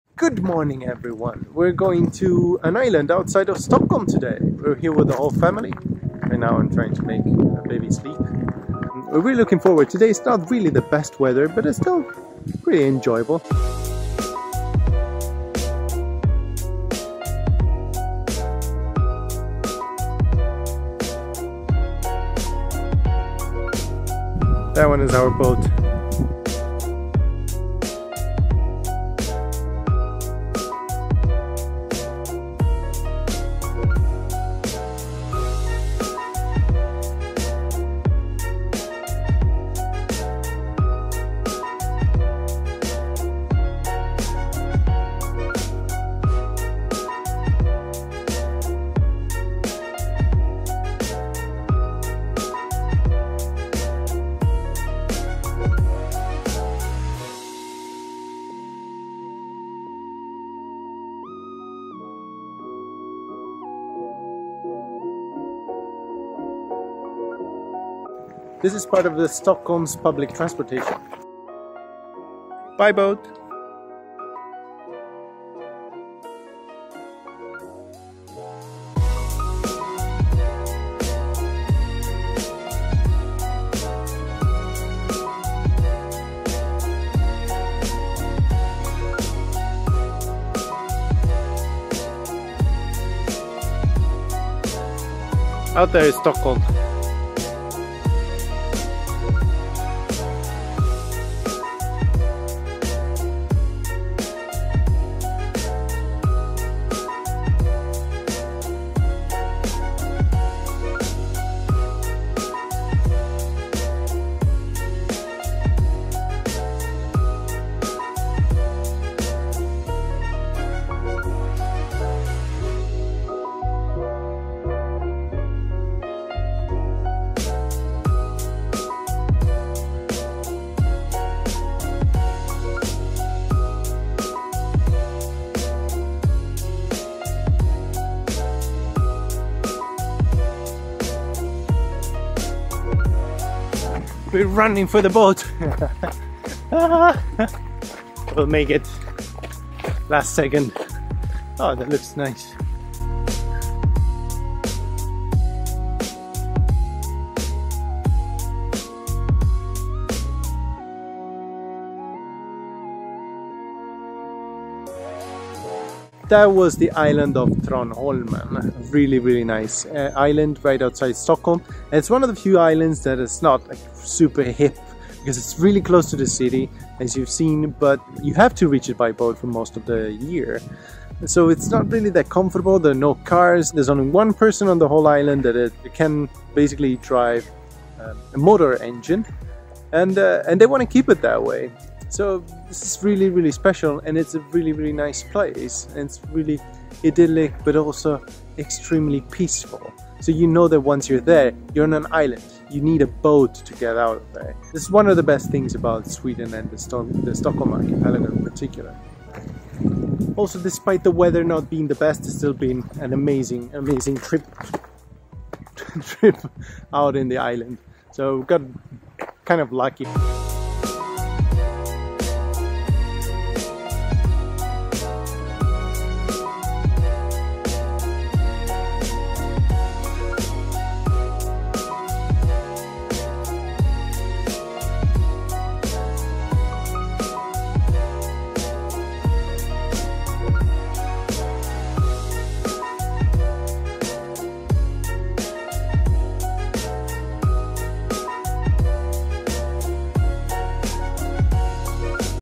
A day trip in the Stockholm archipelago - the island of Tranholmen - Vlog #13
The island of Tranholmen is a unique residential island outside Stockholm. It can be reached by commuter ferry - or by a temporary pedestrian bridge during winter - but no cars or scooters are allowed on the island!

Today we visit a good friend who lives there!